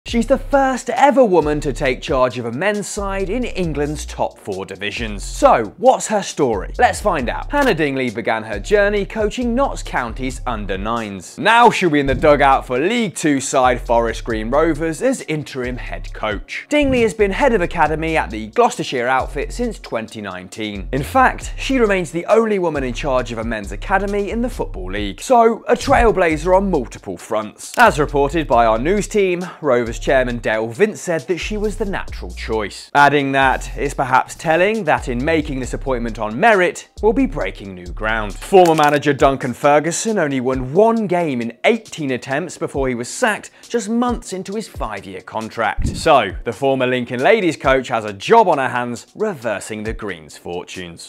First Woman Manager Appointed In Men's Football 🚨
Welcome to The Athletic FC, where quality football journalism meets bitesize video.

In this episode, we breakdown Hannah Dingley's seminal appointment as interim head coach at Forest Green Rovers.

She's now the first ever woman to take charge of a men’s side in England’s top four divisions.